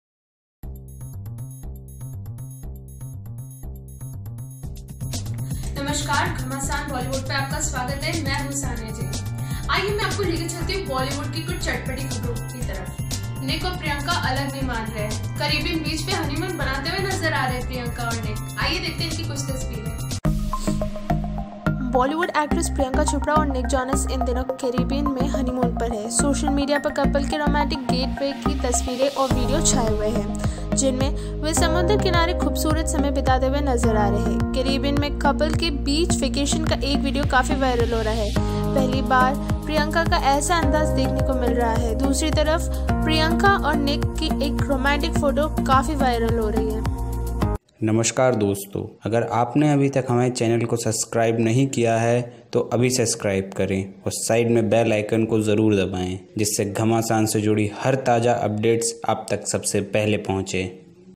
Enjoying Honeymoon ! हनीमून एन्जॉय कर रहे हैं निक-प्रियंका (Nick Priyanka), वीडियो आया सामने
Nick Jonas - Priyanka Chopra (Honeymoon) बॉलीवुड एक्ट्रेस प्रियंका चोपड़ा और निक जोनस इन दिनो..